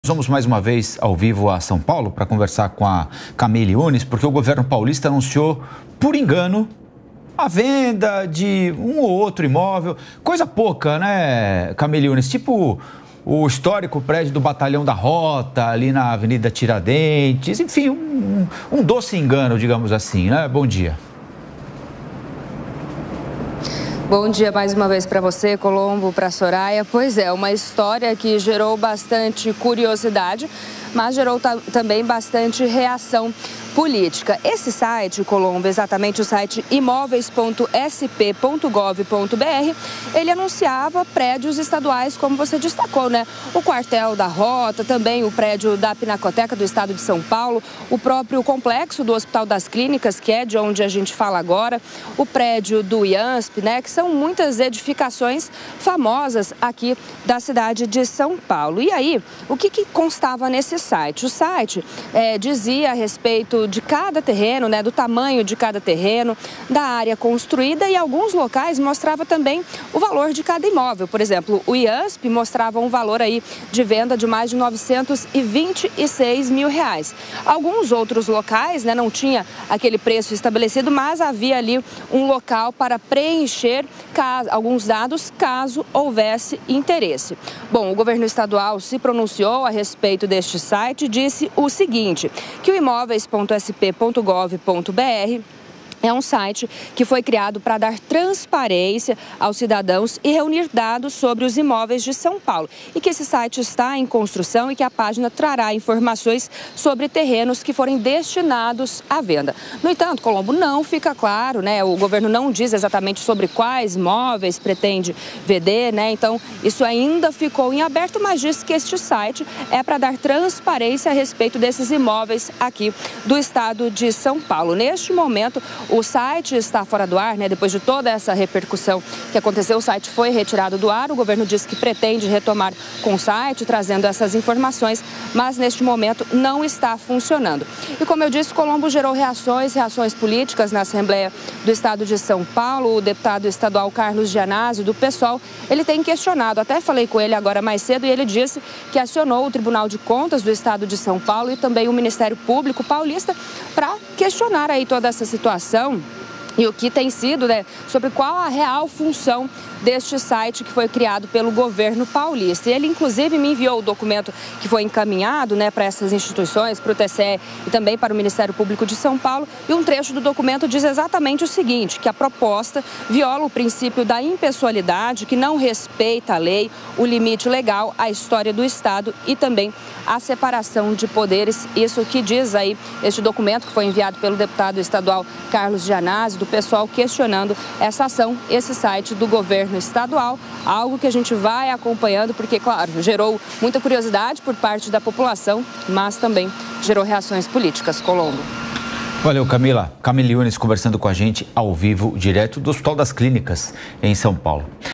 Governo de SP anuncia venda de prédios históricos por engano
O governo paulista anunciou por engano a venda de imóveis de grande valor patrimonial e histórico, como o prédio da Pinacoteca, o batalhão da Rota e o complexo do Hospital das Clínicas. O site "Imóveis.sp.gov.br" foi criado como um serviço de prestação de contas, mas, de acordo com o representante do governo, houve uma falha técnica e o mesmo foi tirado do ar.